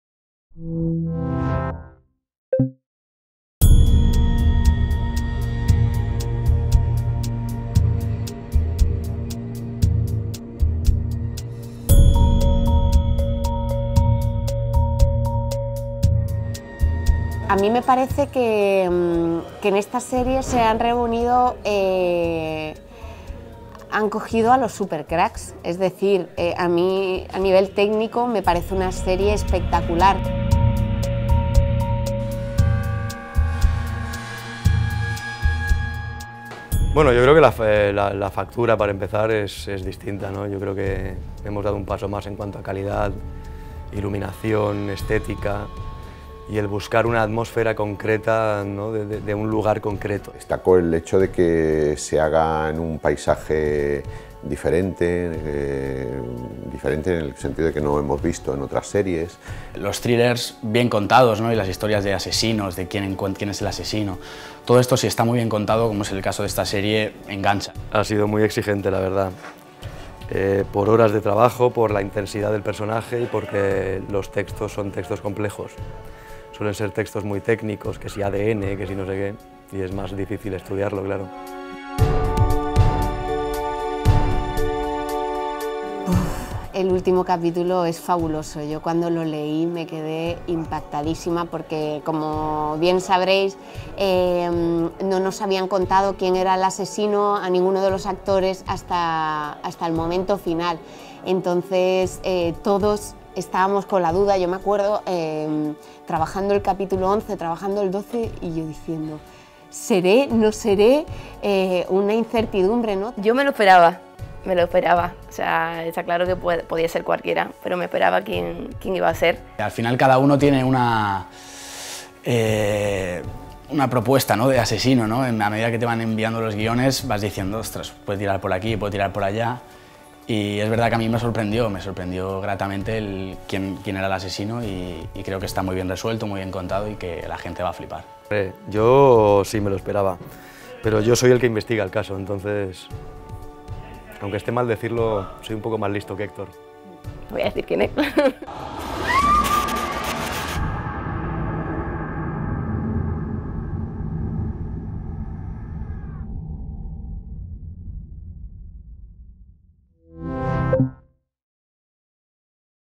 Mar de Plástico: Conoce a los actores y todos sus secretos
Descubre todos los secretos de Mar de Plástico, la serie que causa furor en la televisión Disfruta en este vídeo de las declaraciones de Rodolfo Sancho, Lisi Linder, Pedro Casablanc o Patrick Criado.